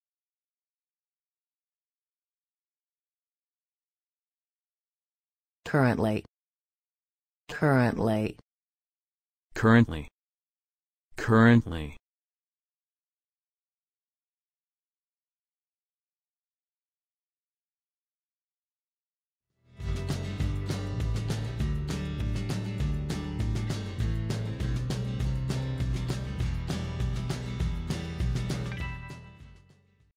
वर्तमान में इंग्लिश में क्या है
वर्तमान में को अंग्रेजी में सही तरह से कैसे बोलें। ध्यान से सुनो।
वर्तमान में का हिंदी से अंग्रेजी में सही अनुवाद क्या है?आप अमेरिकी व्यक्ति की यरह बोलिए।